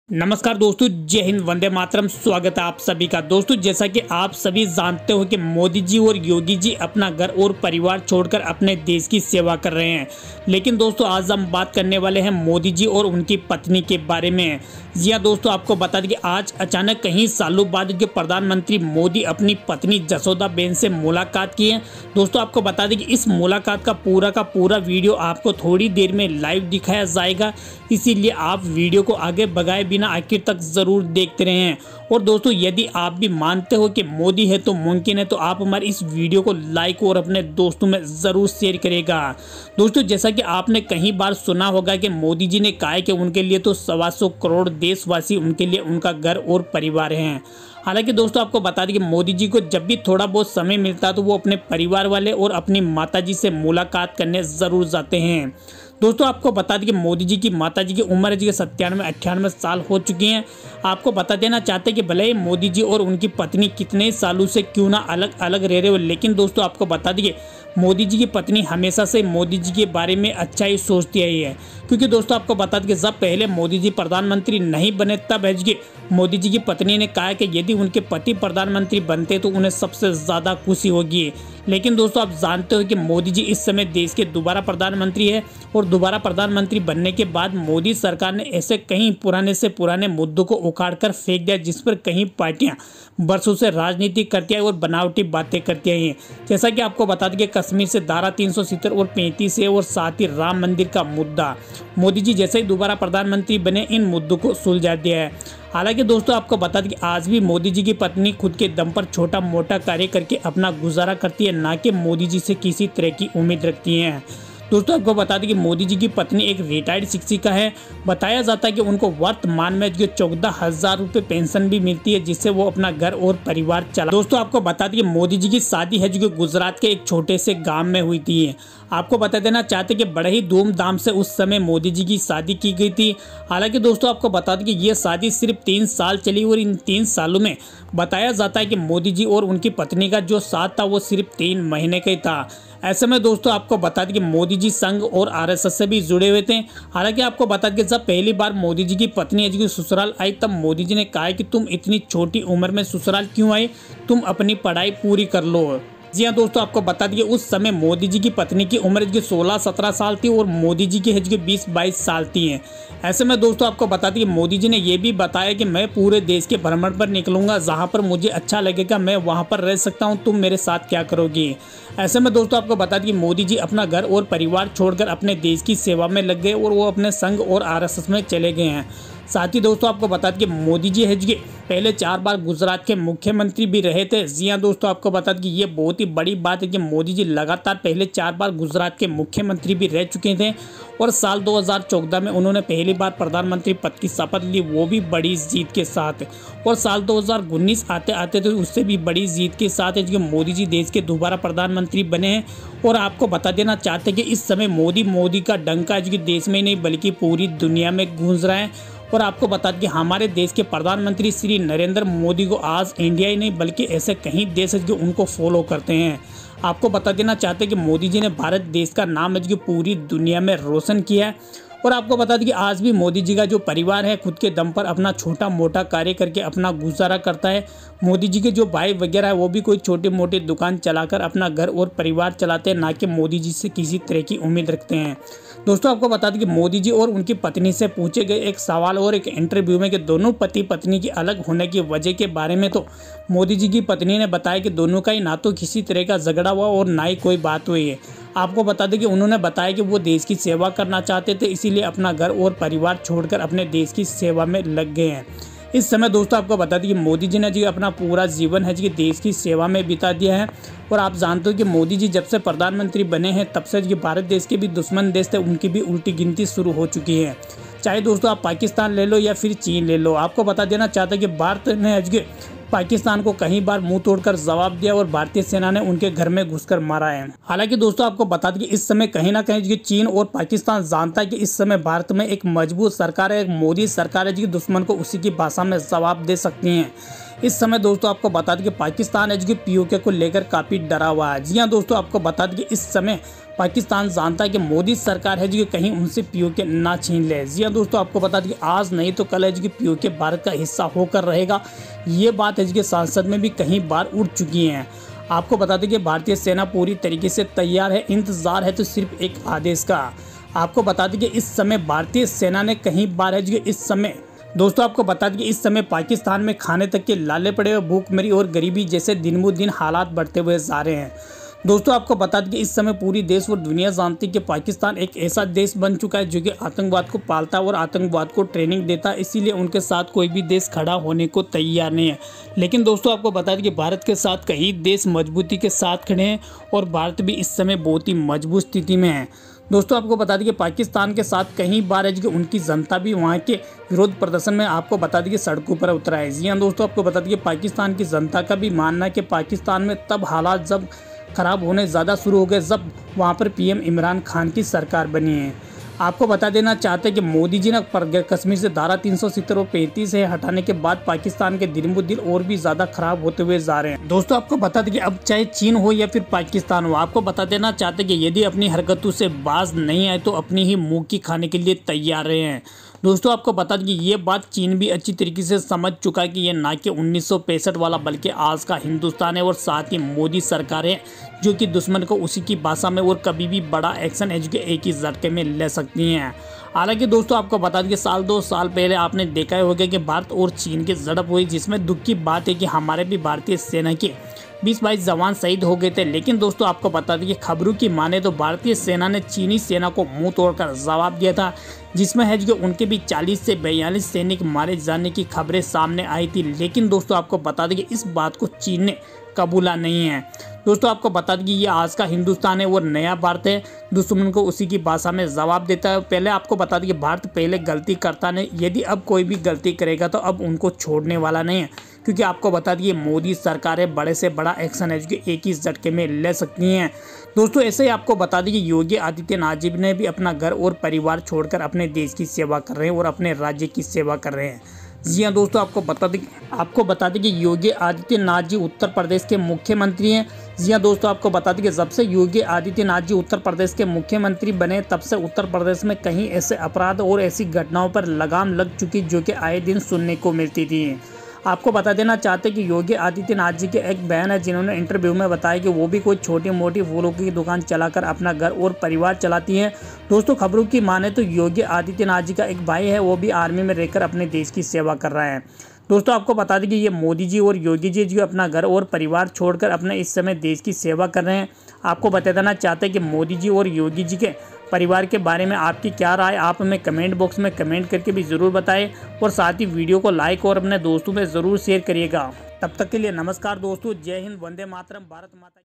देखिए प्रधानमंत्री मोदी अपनी पत्नी जशोदाबेन से मिलने पहुंचे
मोदी पत्नी जशोदाबेन
पीएम मोदी मां हीराबेन